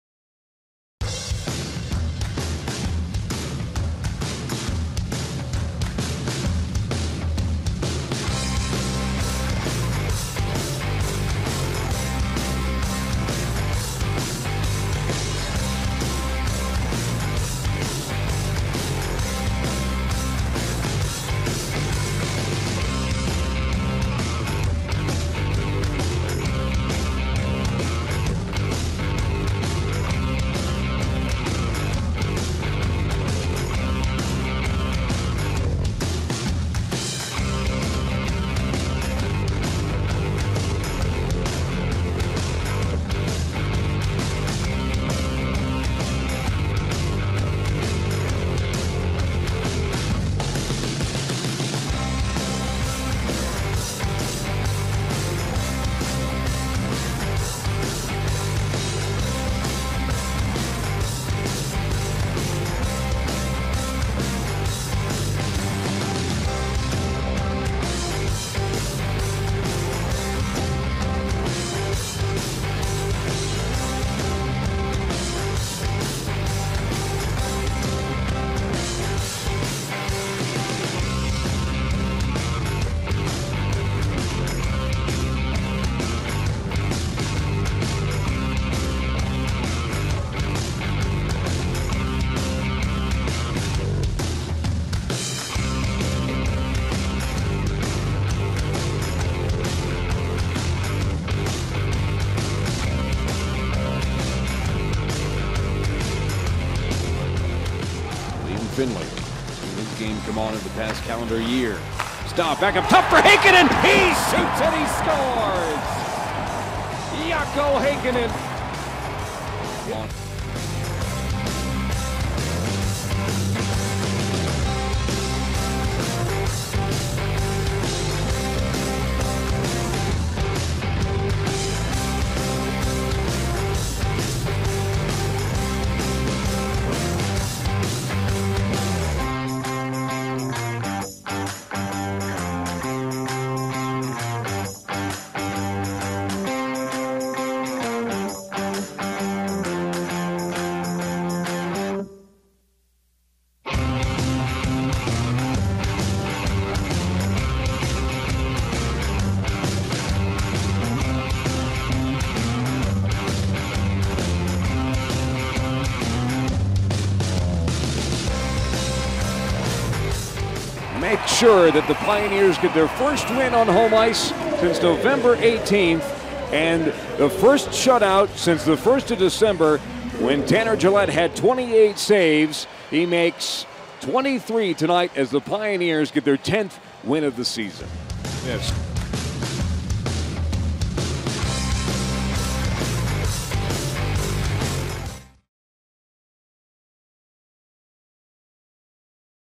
HKY v Dartmouth (15 DEC 17) - Highlights
HIGHLIGHTS - Tanner Jaillet made 23 saves en route to the University of Denver Pioneers' (10-3-4, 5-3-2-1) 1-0 victory against the Dartmouth College Big Green (2-8-1, 2-4-1) on Friday at Magness Arena.